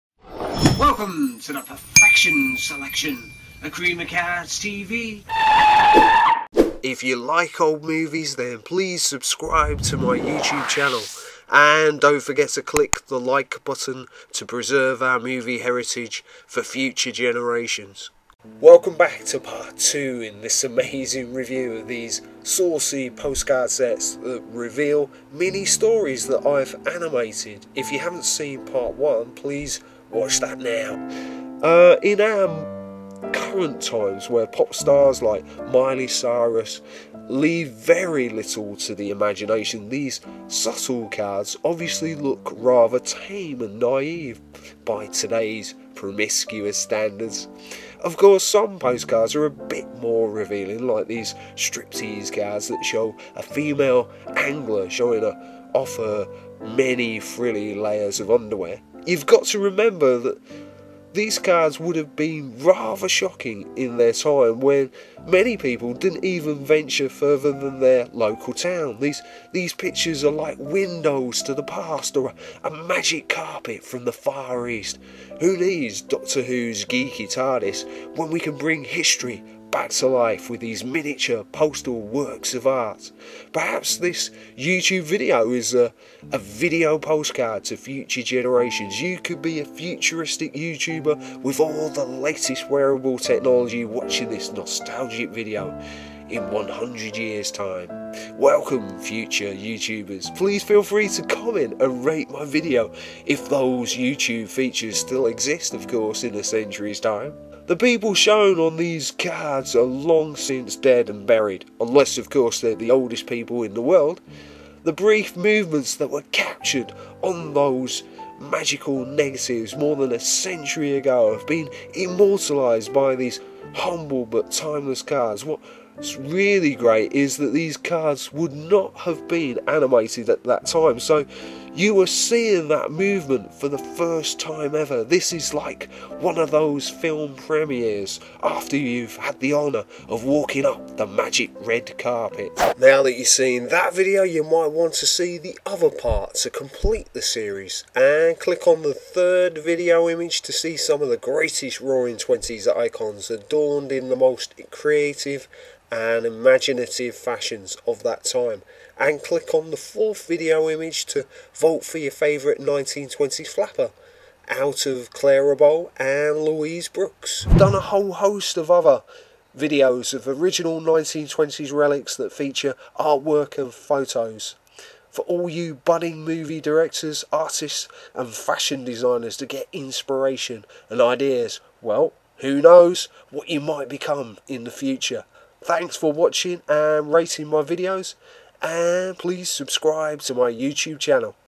Saucy Old Postcards 1900s-1910s Pt2 Vintage Erotica
What shocking things did the butler see? Find out what shocked people in the early 1900s.